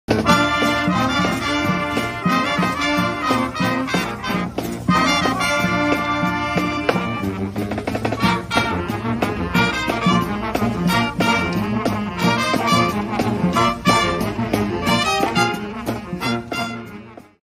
Chatsworth Charter High School Cheerleaders
It was an honor to have the @Chatsworth Marching Chancellors open up our BD3Fest! Under James Choi's direction, this group won numerous awards, including 2nd place at LAUSD Marching Band Championship!